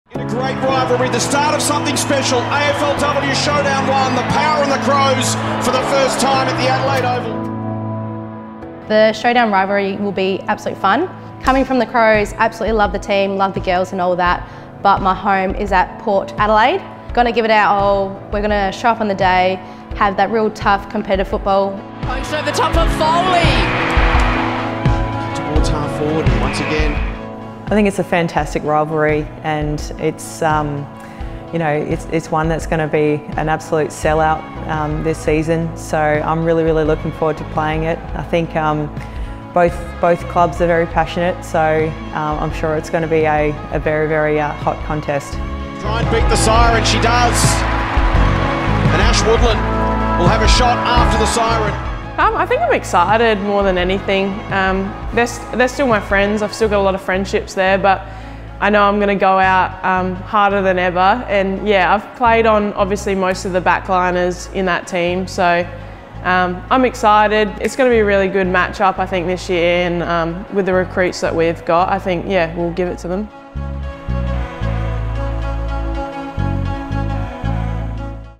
SHOWDOWN II | What it means 👊
Justine Mules, Erin Phillips and Ash Saint speak on the feeling heading into a Showdown.